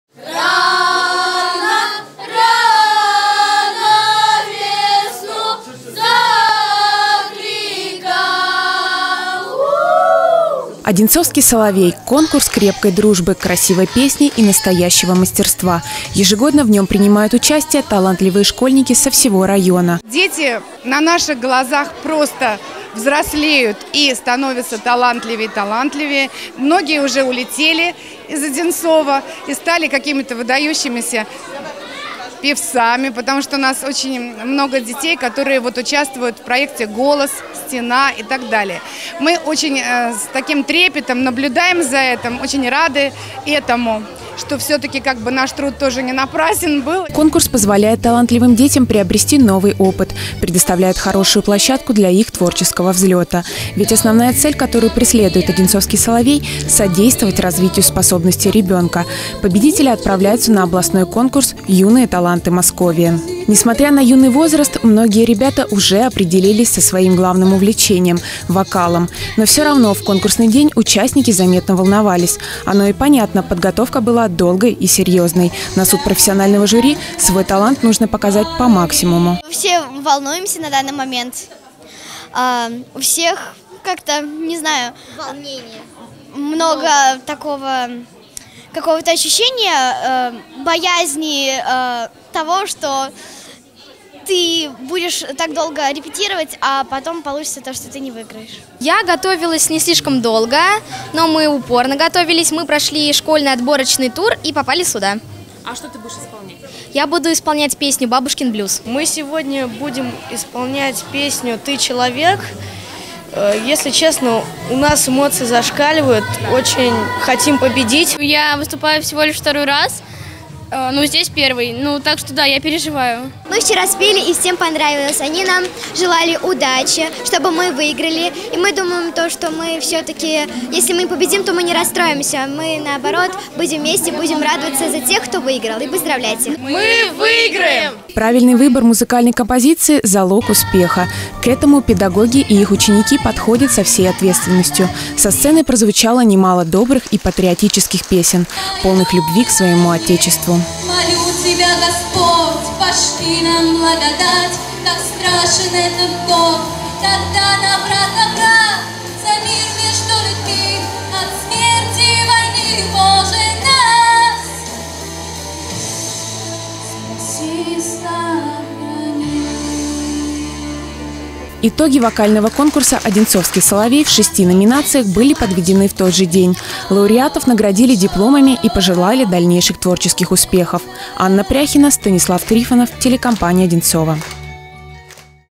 Вокальный конкурс «Одинцовский соловей» состоялся в центре эстетического воспитания
В минувшую субботу в Одинцовском центре эстетического воспитания прошел вокальный конкурс «Одинцовский соловей». Этот ежегодный праздник песни традиционно собирает таланты со всего района. Конкурс в рамках фестиваля «Ступени» прошёл уже в десятый раз. В вокальном состязании приняли участие 425 солистов из 40 школ и 2-х учреждений дополнительного образования. Наша съёмочная группа побывала на выступлении юных дарований.